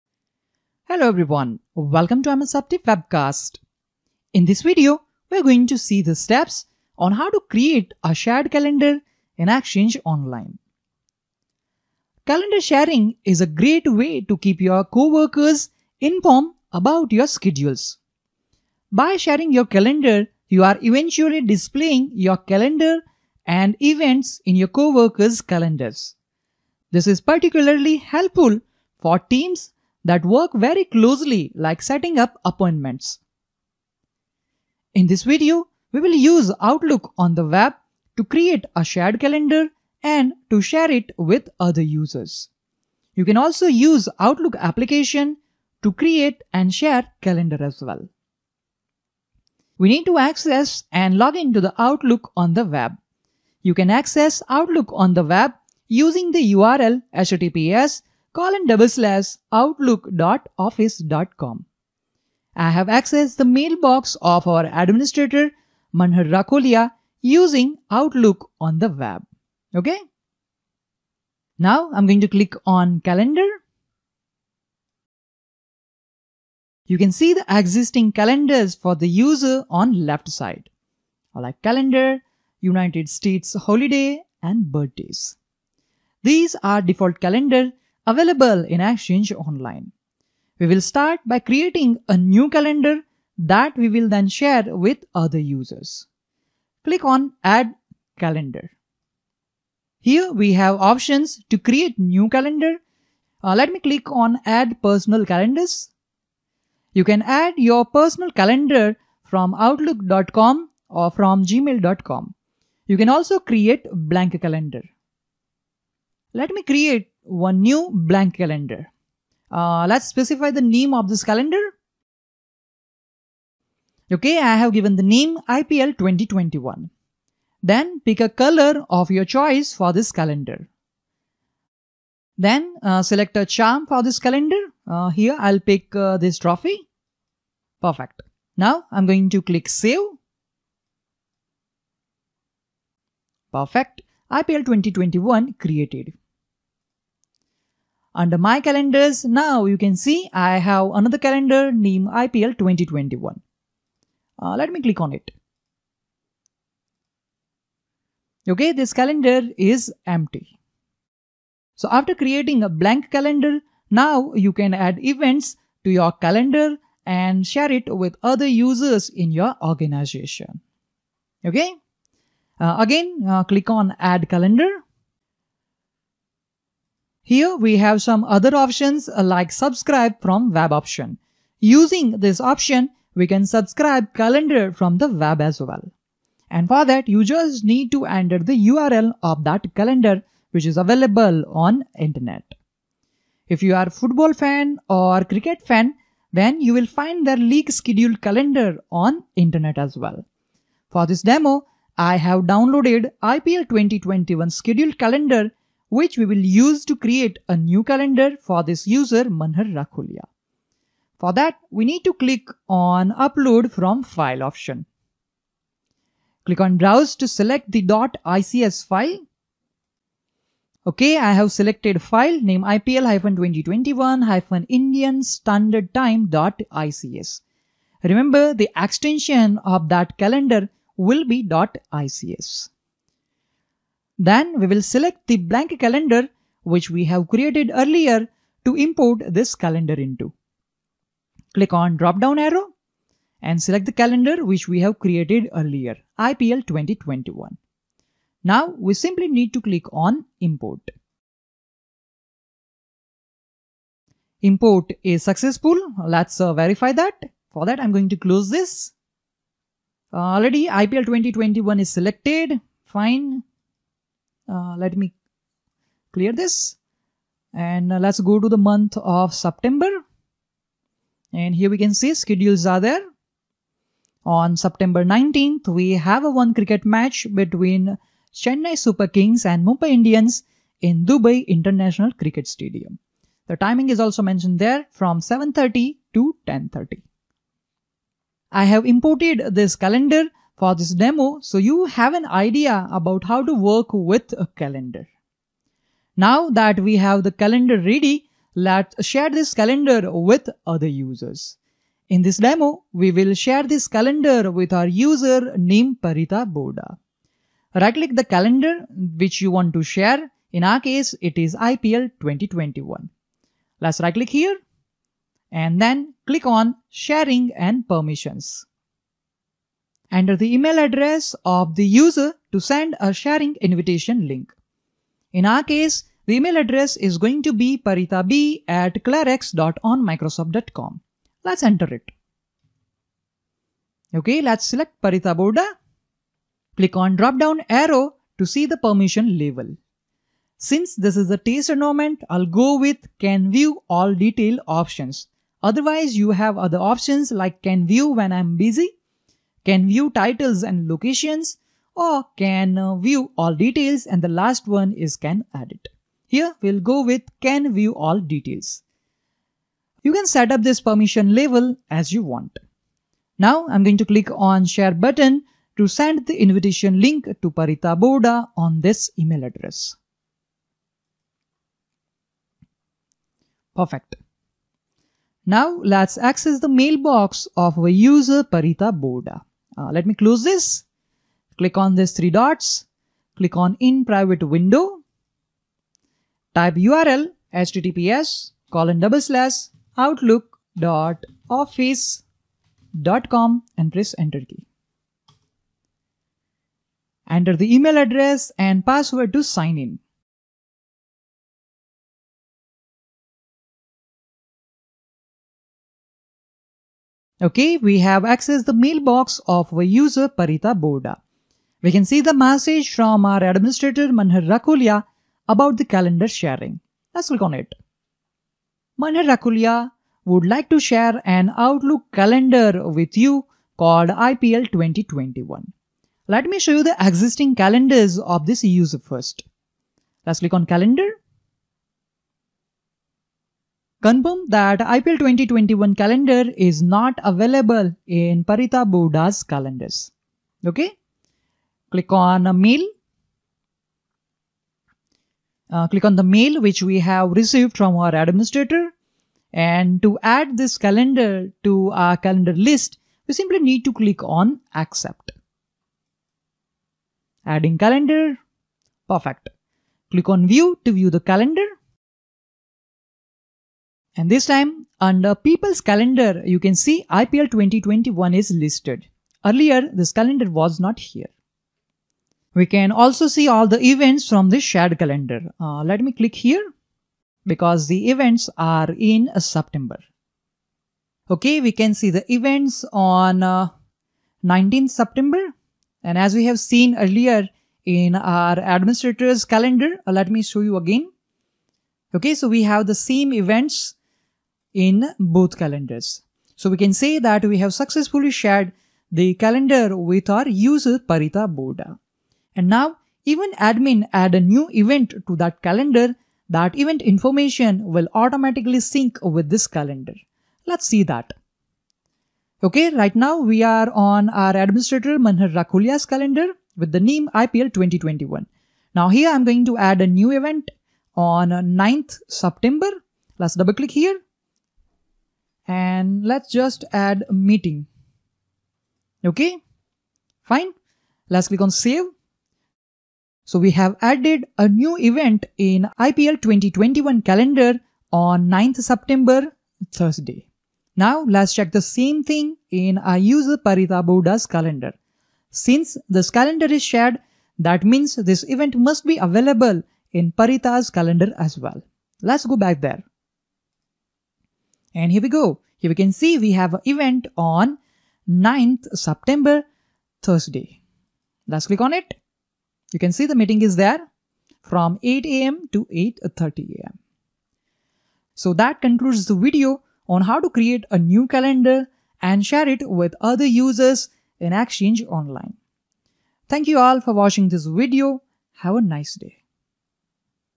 33. How to Create a Shared Calendar in Microsoft 365 | Exchange Online
Microsoft 365 Exchange Online Video Series:

This is a step by step video on How to Create a Shared Calendar in Microsoft 365 using OWA (Outlook on the web) web interface. If you have an Exchange, Microsoft 365, or Outlook.com account, you can send a sharing invitation to other users so they can view your calendar in their own Calendar list.
You can create a calendar folder for a specific project and share it with your co-workers. Optionally, you can grant them permission to modify the events and appointments in the calendar.